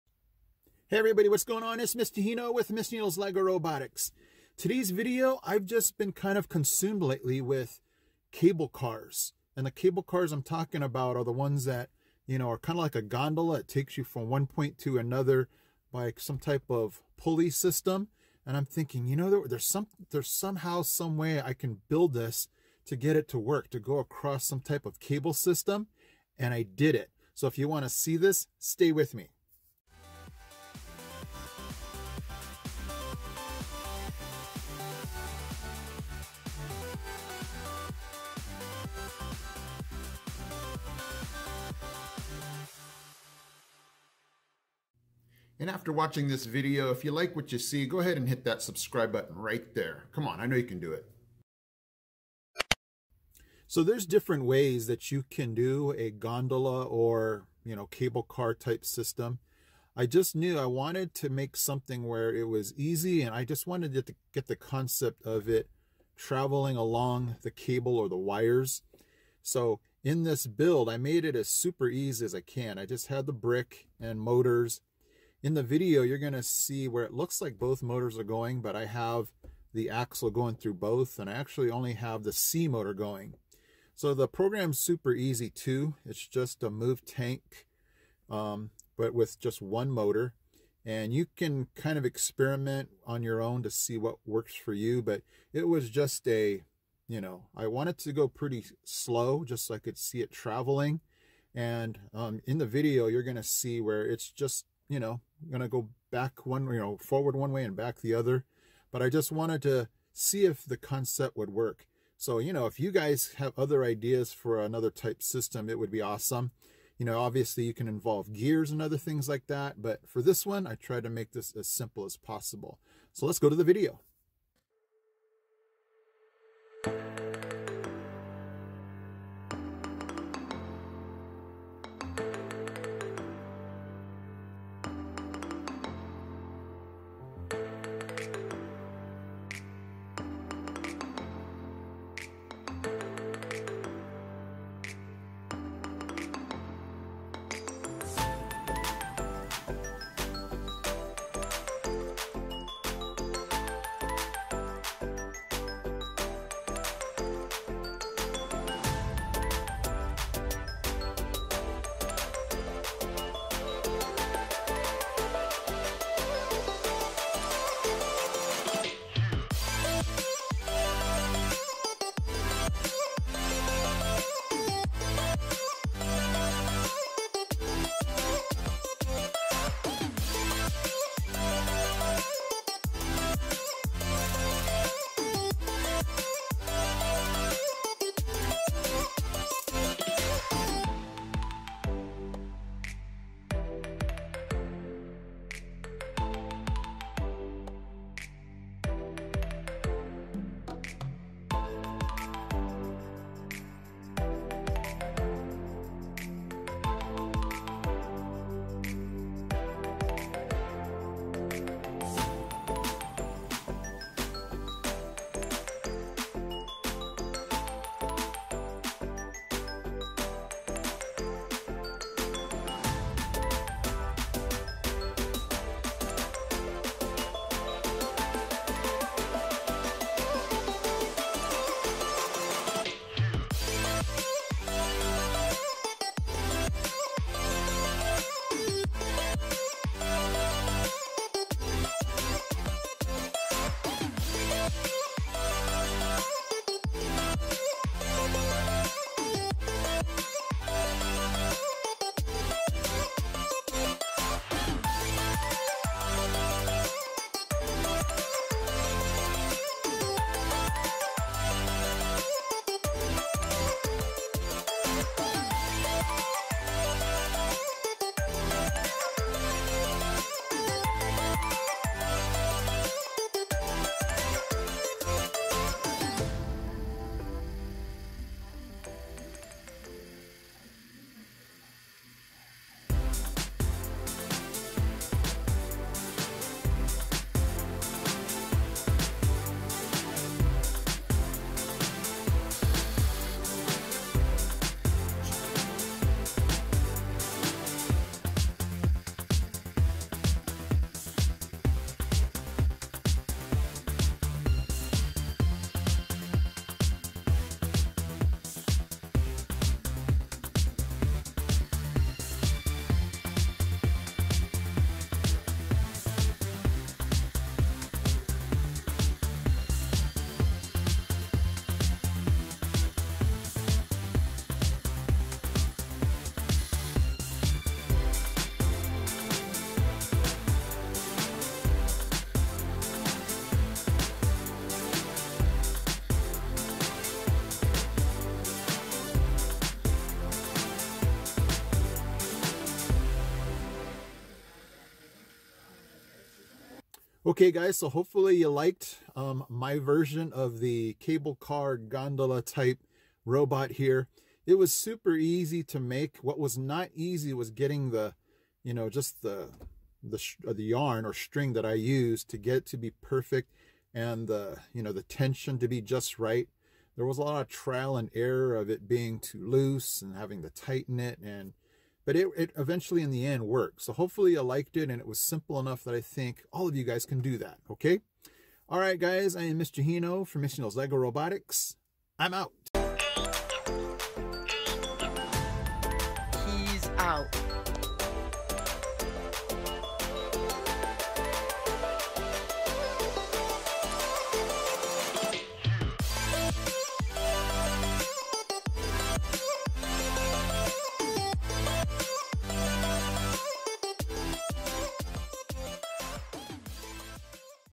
"Mr. Hino's LEGO Mindstorms EV3 Gondola Cable Car!!"
Hey guys!!! I wanted to create some type of Gondola or Cable Car using the LEGO Mindstorms EV3 Kit and here it is!!  It's not the most technical or sophisticated build, but it accomplished what I wanted... a transportation vehicle that moved along cables (or yarn in my case).  Hope you enjoy!!